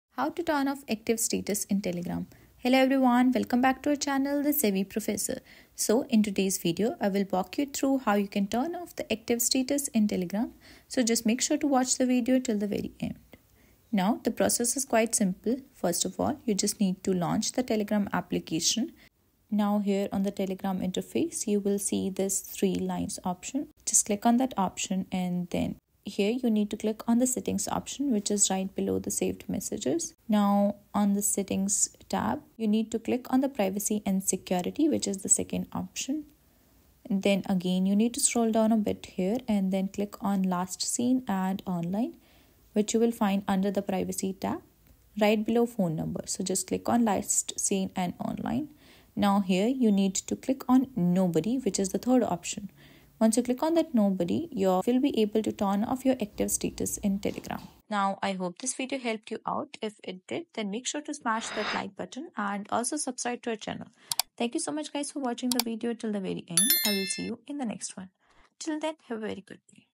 How To Turn Off Active Status In Telegram (How To Hide Last Seen And Online Status On Telegram)
How To Turn Off Active Status In Telegram (How To Hide Last Seen And Online Status On Telegram). In this video tutorial I will show how to turn off Active Status in Telegram.